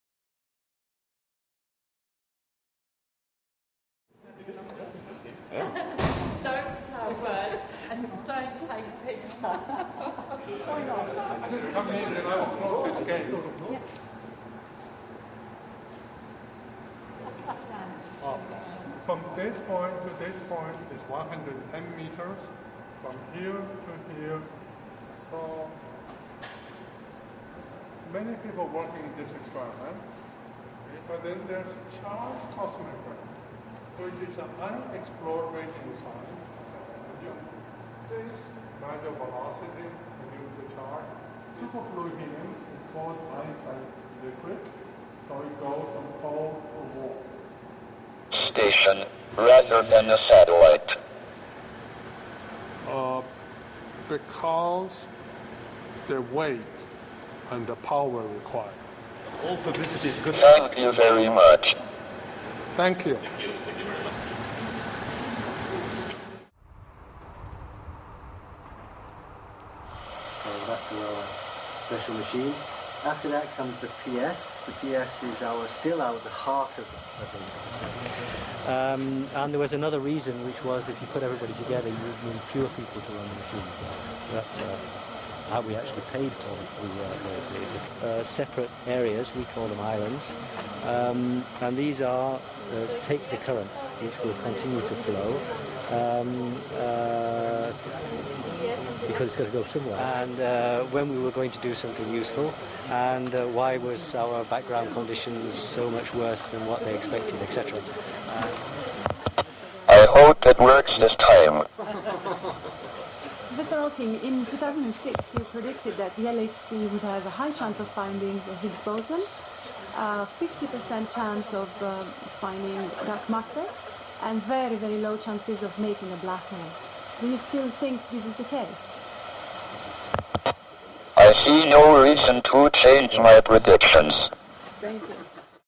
Stephen Hawking visit to CERN
Highlights of Stephen Hawking visit to CERN (18/09/2009)
Les points forts de la visite de Stephen Hawking au CERN (18/09/2009)
Resumen de la visita de Stephen Hawking  al CERN en Ginebra.